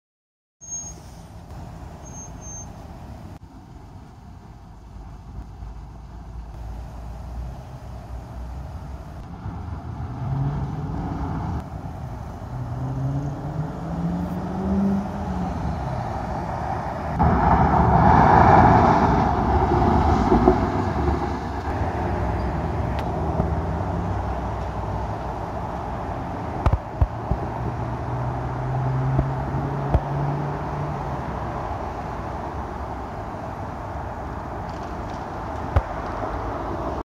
20200123 164240 V-8 Dodge Challenger 392 Muscle car
Shaker-hood HEMI V-8 Dodge Challenger 392 acceleration visual. Autogespot production. Share on Facebook, Pinterest, Twitter, Tumblr and Instagram.